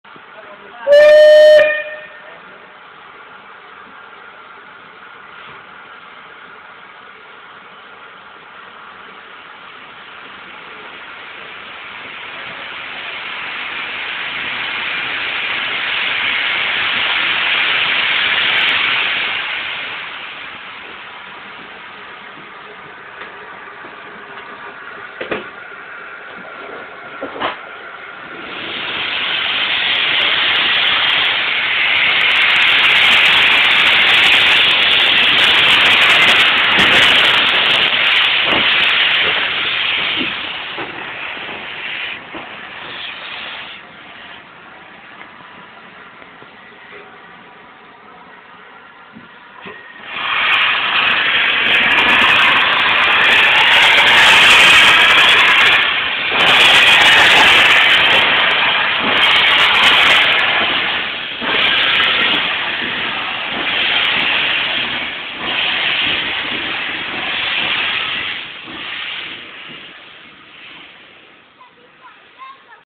34016 Bodmin
34016 comes of the pit road at ropley for a countryman dinning train working